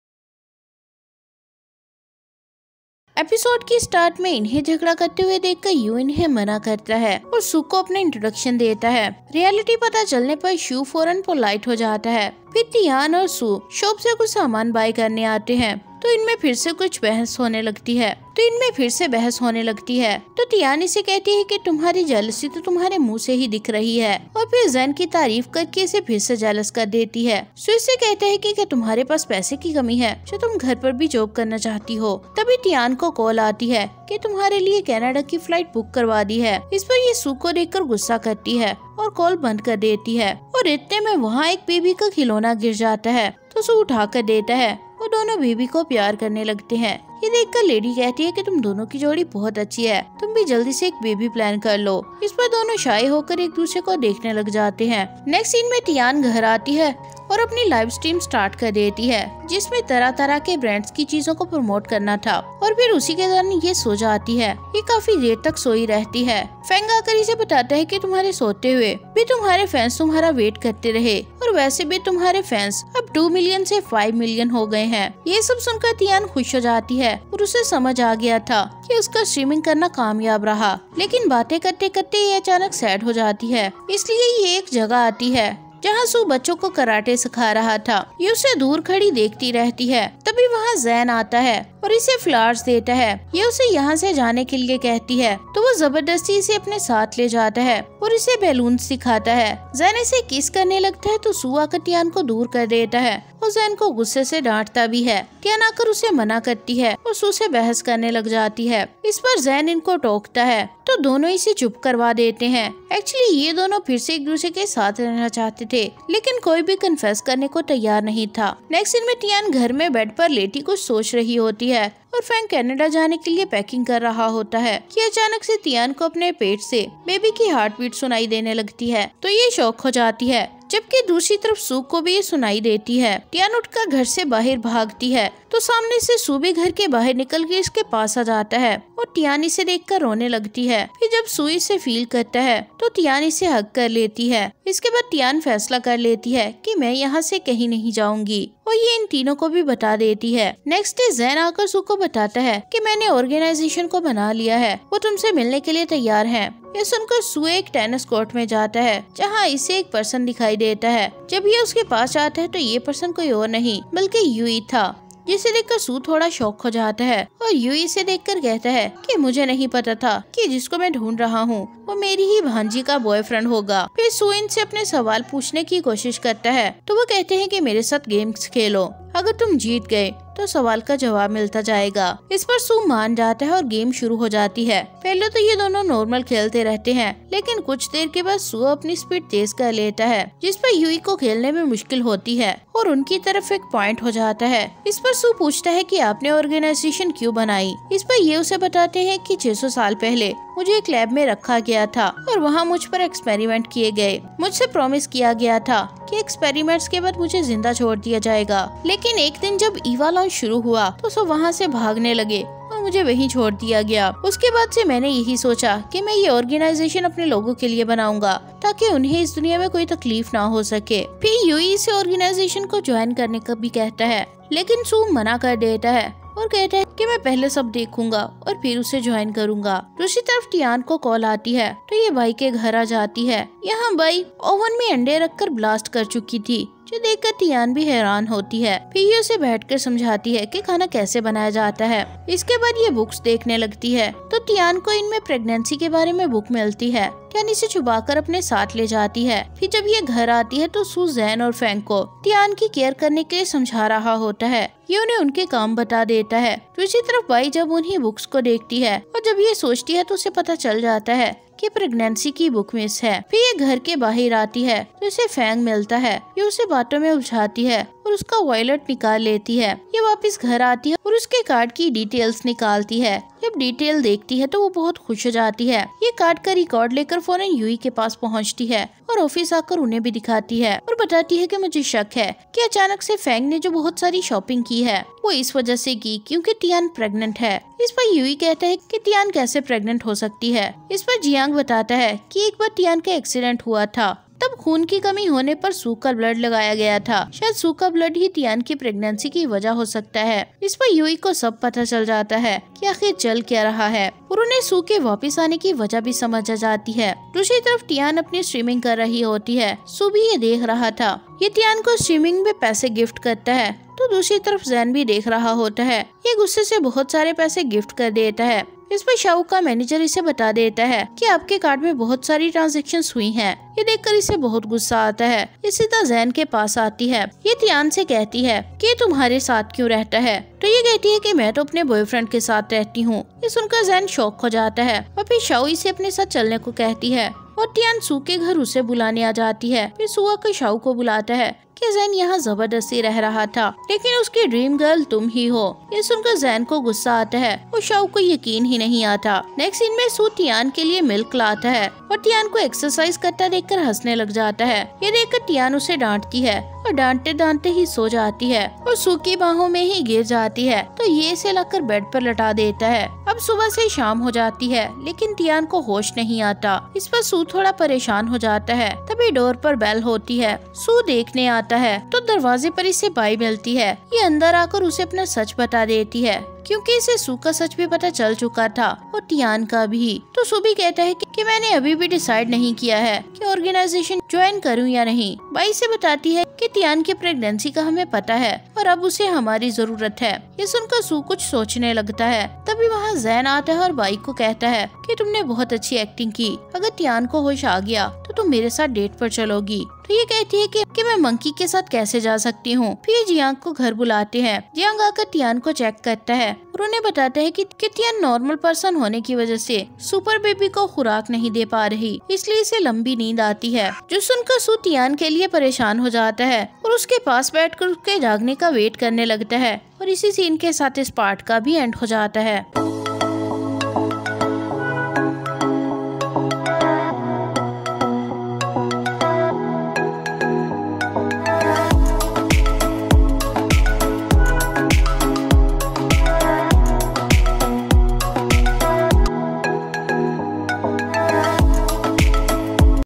Hot Demon is Worried About his Pregnant Ex-Girlfriend || Part 5 || Chinese Drama Explained in Hindi
Hot Demon doesn't know He has A Cute Son with Ex-Girlfriend Part 4| Chinese Drama Explained in Hindi